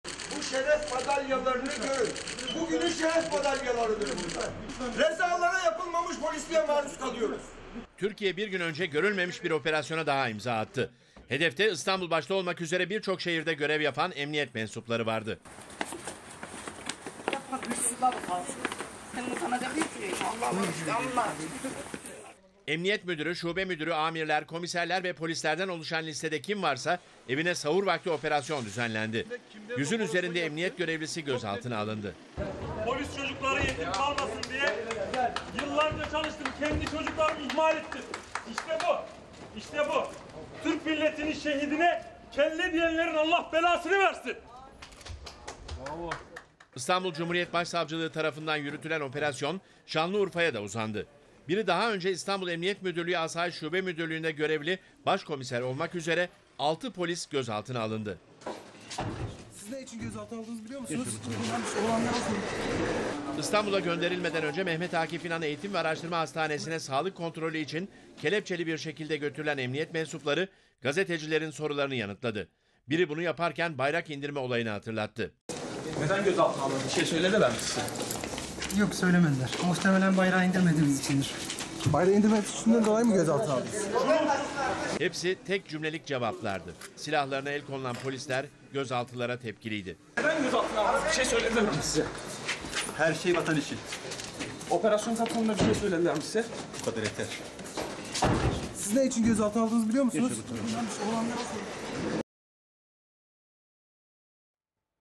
BAYRAK İNDİRMEDİK DİYE ALDILAR
Emniyet mensuplarına İstanbul merkezli başlayan baskınlar, Şanlıurfa'ya da sıçradı. Şanlıurfa'da biri başkomiser 6 polis memuru gözaltına alındı. Gözaltına alınanlardan başkomiser Erkan Ünal sağlık kontrolünden çıkarken operasyonu bakın nasıl yorumladı.